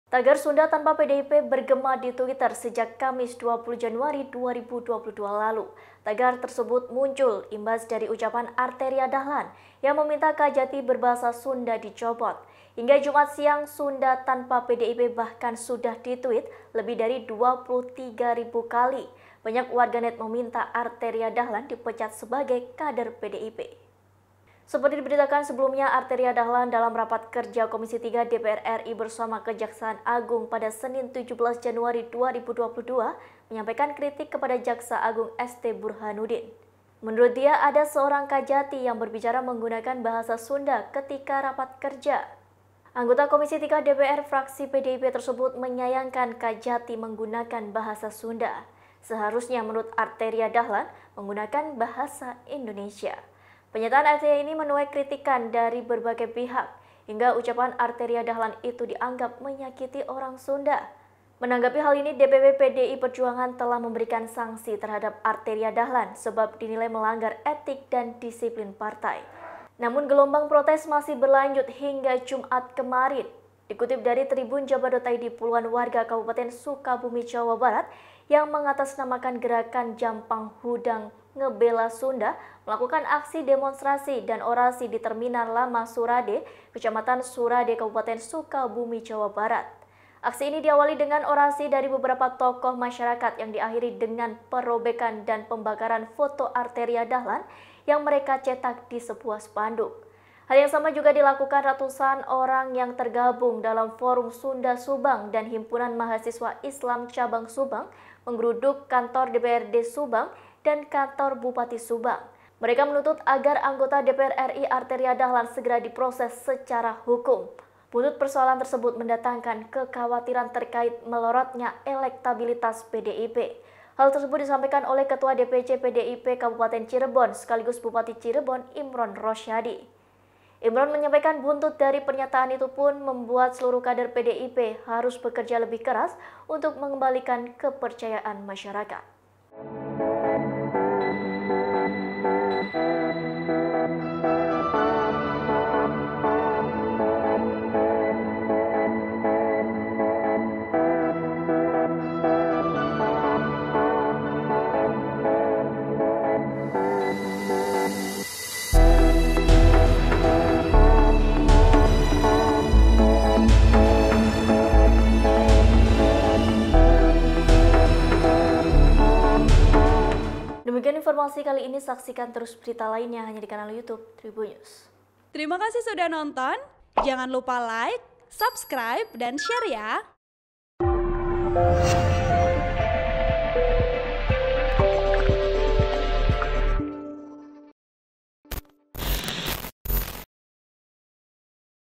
Ramai Tagar SundaTanpaPDIP Buntut Ucapan Arteria Dahlan, Akankan Pengaruhi Suara PDIP di Jabar?
TRIBUN-VIDEO.COM - Tagar SundaTanpaPDIP bergema di Twitter sejak Kamis (20/1/2022).

Tagar tersebut muncul imbas dari ucapan Arteria Dahlan yang meminta Kajati berbahasa Sunda dicopot.

Hingga Jumat (21/1/2022) siang, SundaTanpaPDIP bahkan sudah di-tweet lebih dari 23.200 kali.

Banyak warganet meminta Arteria Dahlan dipecat sebagai kader PDIP.

Seperti diberitakan sebelumnya, Arteria Dahlan dalam Rapat Kerja Komisi III DPR RI bersama Kejaksaan Agung pada Senin (17/1/2022) menyampaikan kritik kepada Jaksa Agung ST Burhanuddin.